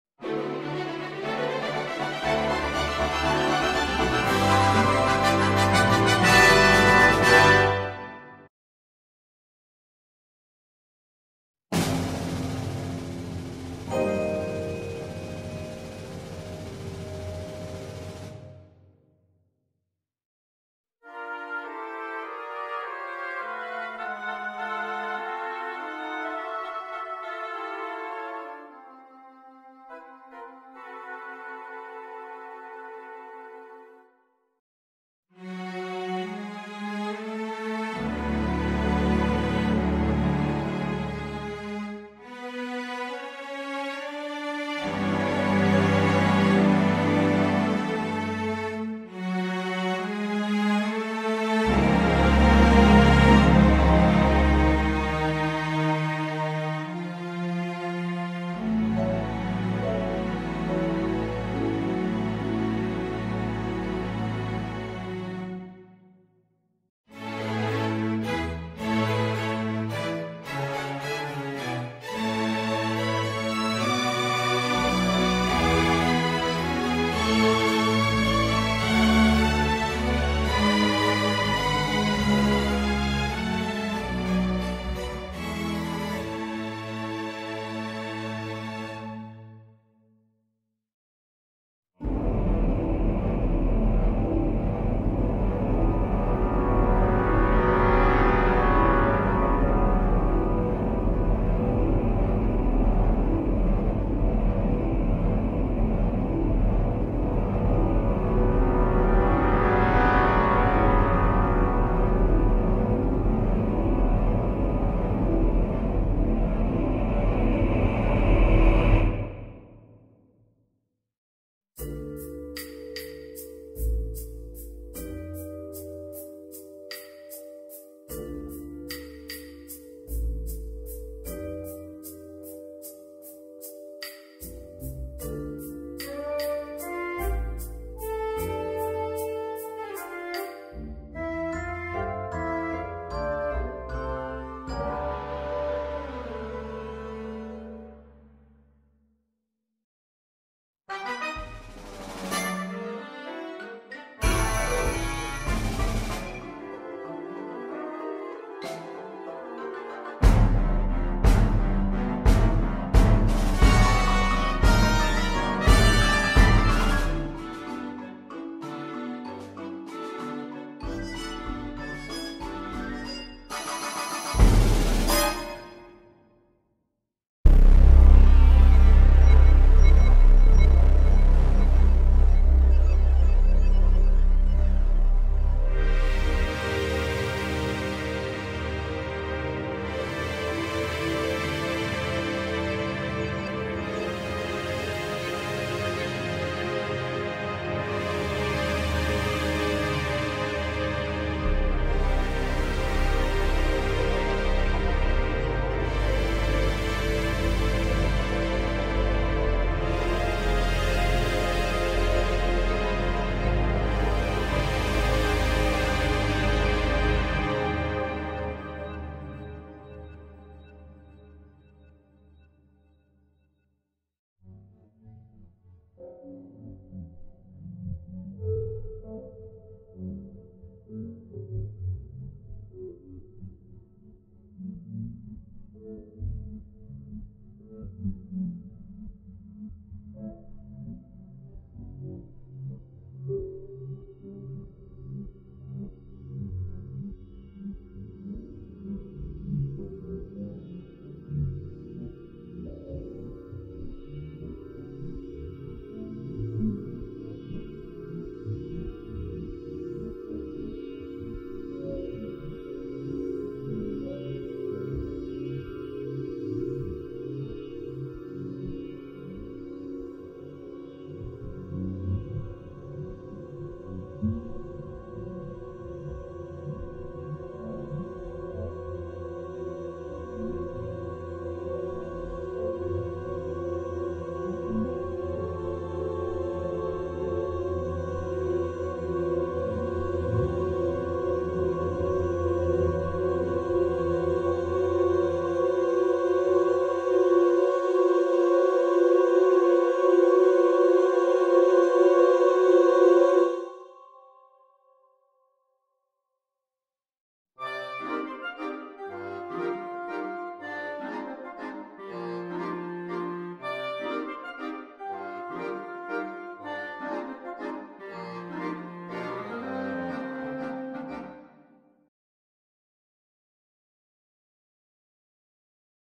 Photo Score Compilation: 1-10
Photo Scores is a project I started on Instagram to create short musical scores to go with interesting pictures. Here is 1-10.